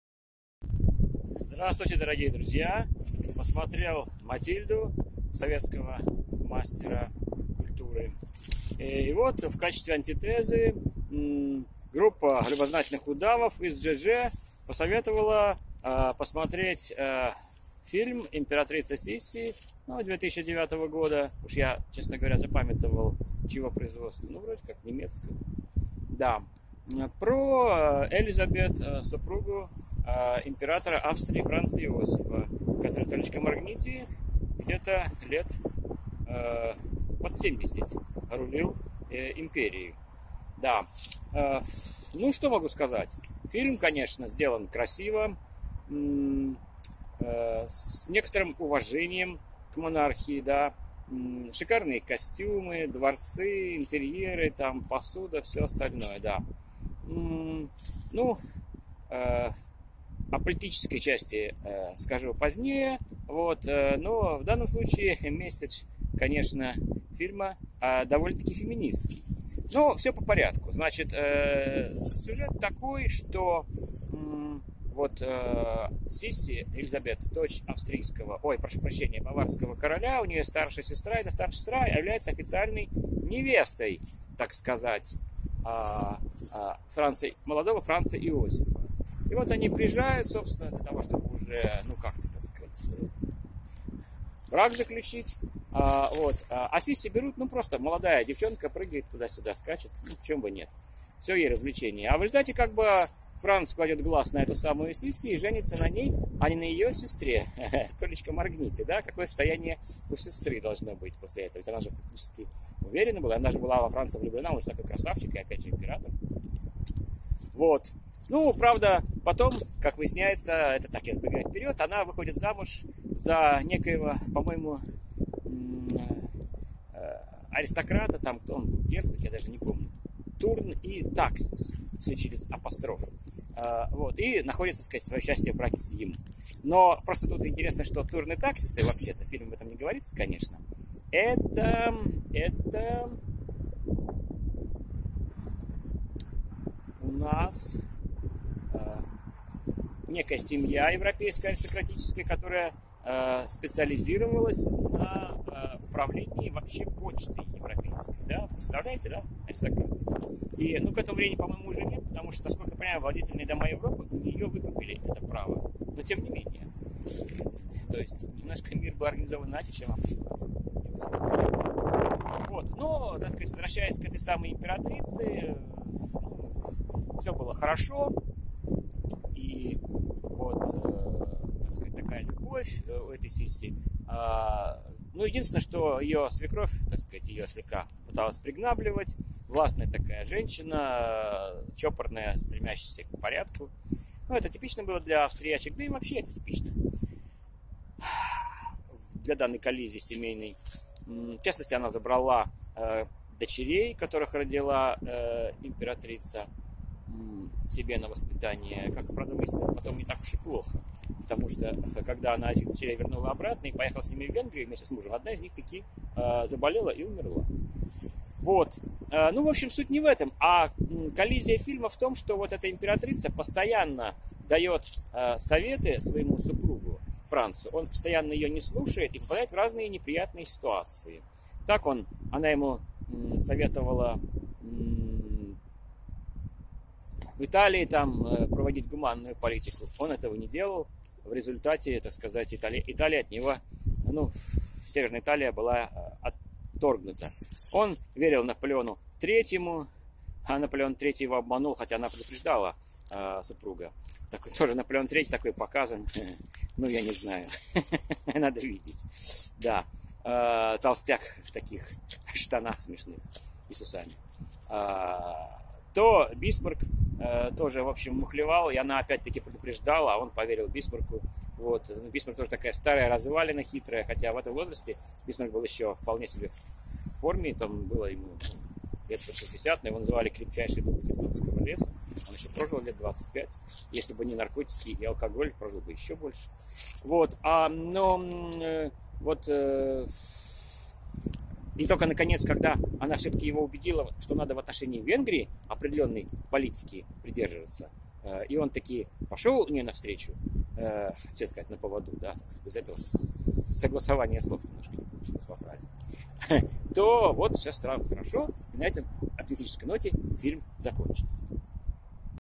«Императрица Сисси», часть I.
Женитьба императора Австрии Франца Иосифа Первого на баварской принцессе Элизабет. Семейные трения. Дипломатия Наполеона Третьего и Бисмарка. Феминизм в фильме.